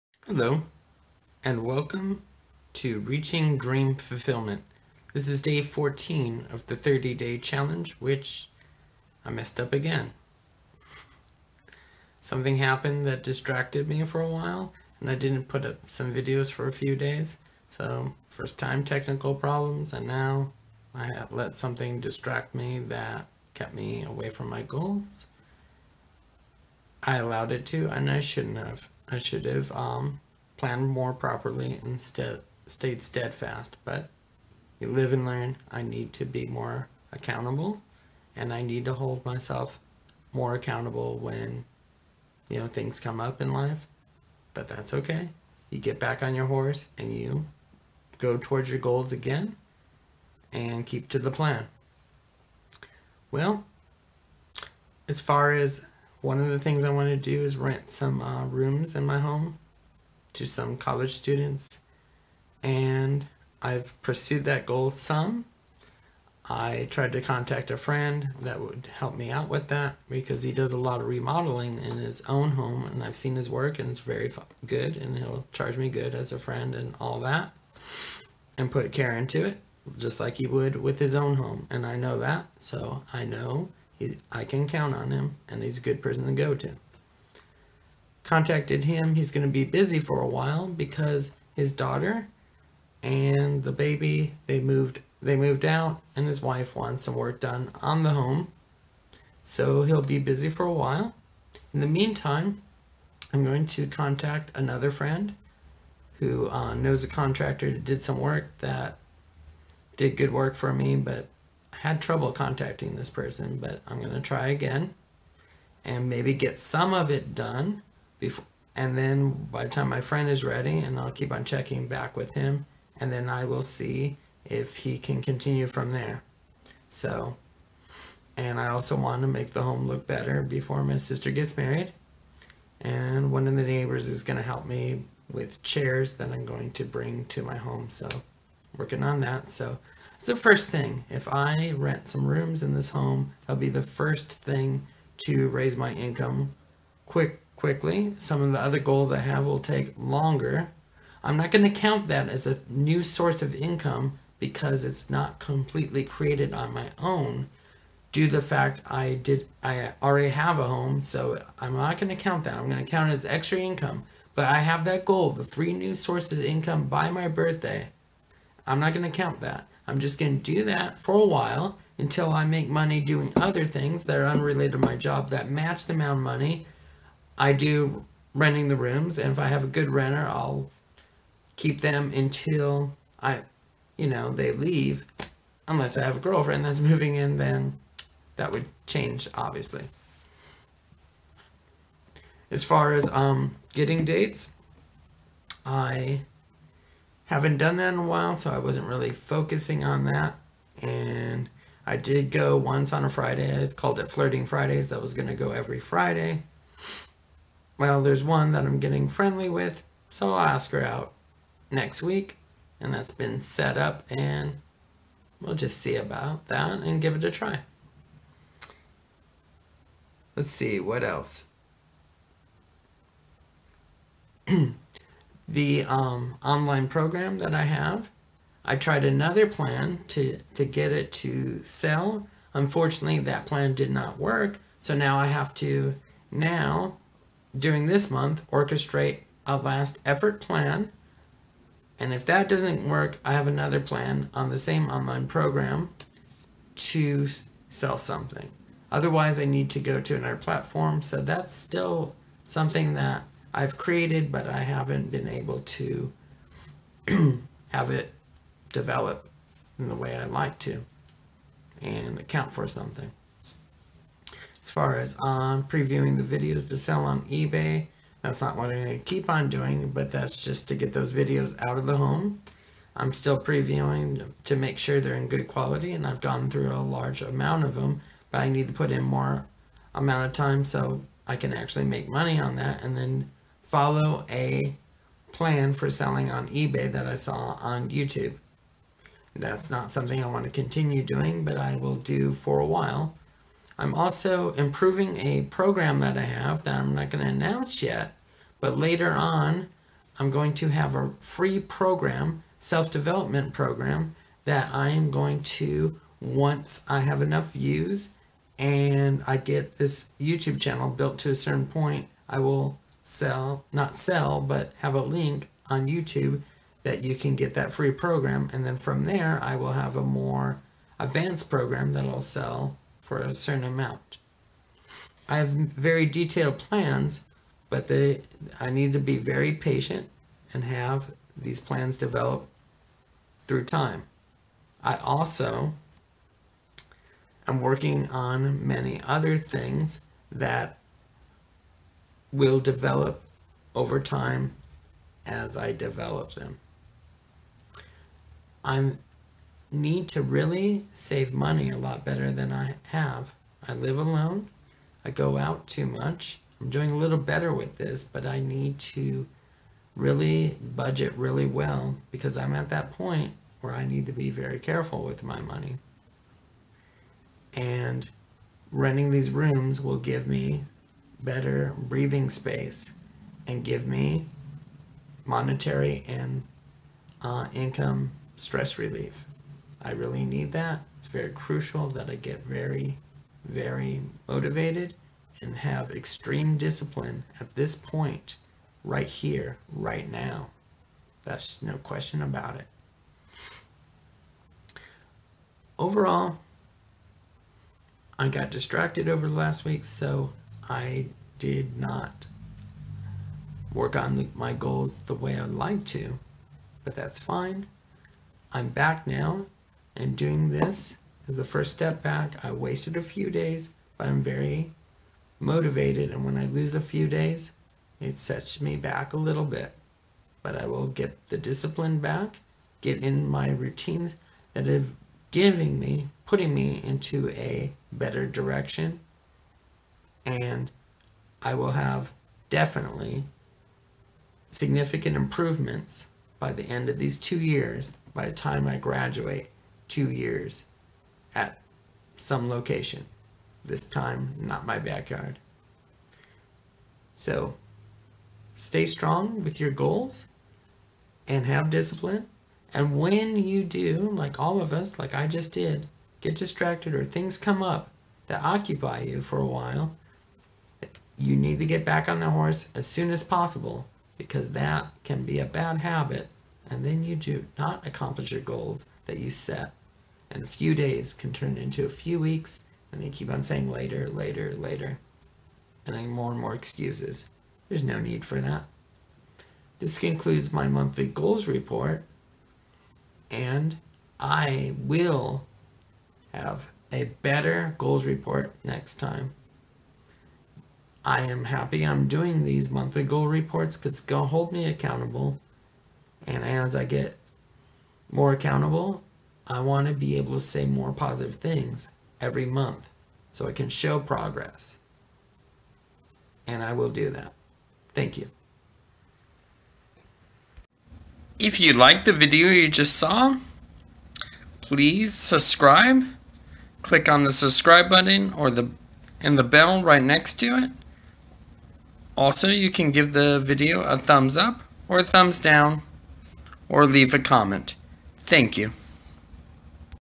monthly goals report September 2017
In this video I give my monthly goals report. I got distracted for a few days and got away from my goals and my discipline. I will get more focused and regain my focus. Discipline is a day to day process.